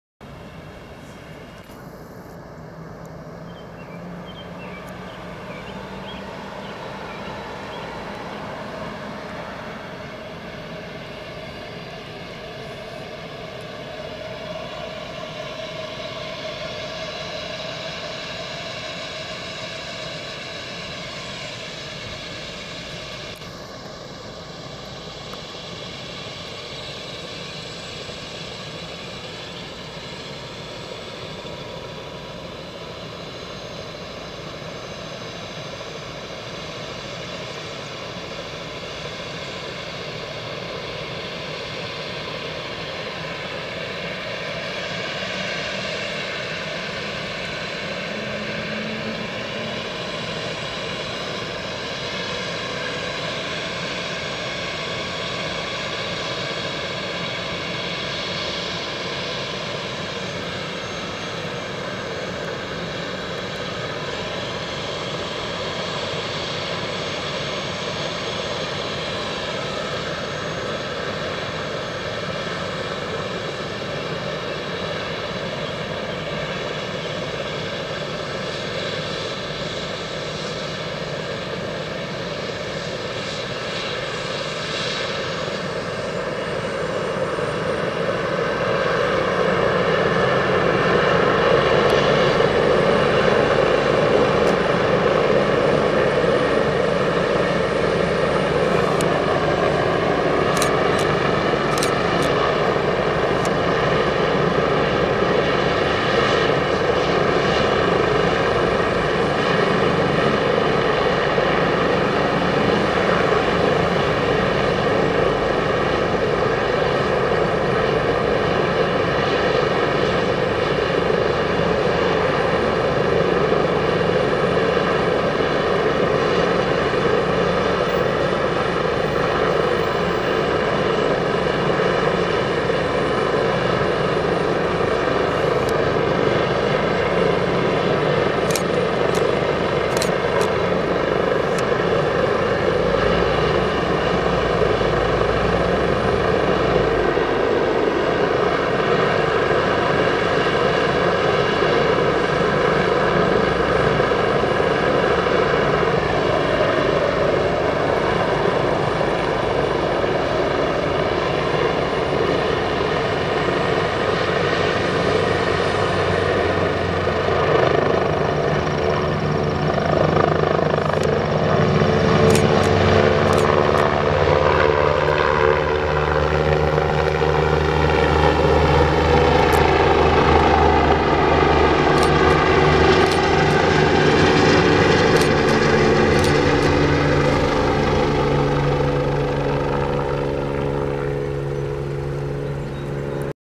PENNSTAR Medevac
PENNSTAR Medevac taking off from Reading Hospital back in May.